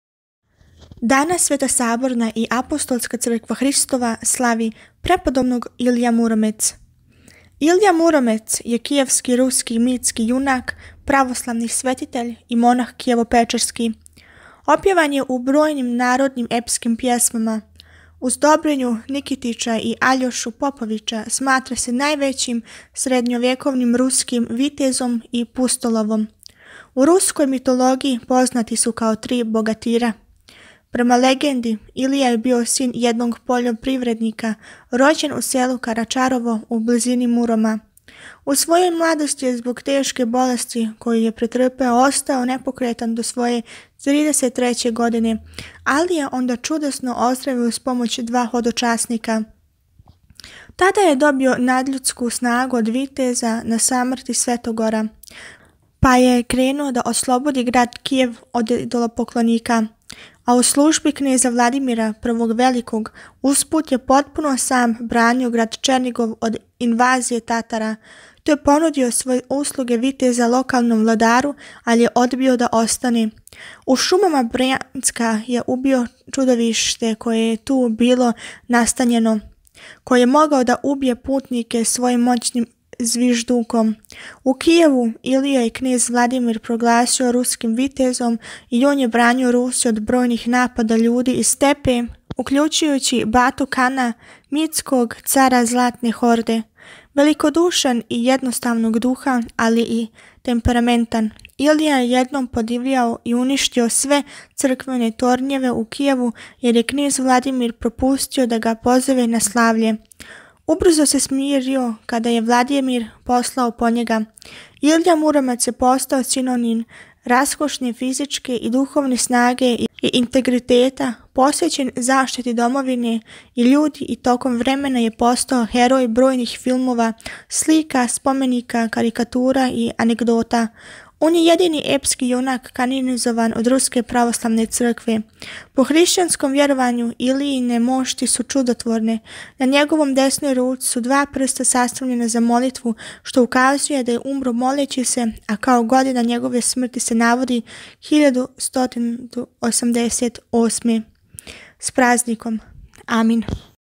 Житије Светих - Преп. Илија Муромец
Данас сд празнује св. Илија Муромец
с празником

"Акатисти за месец Децембар са житијама" - страна 380
Инстагам @hadzi_teodora
ТикТок @hadzi_teodora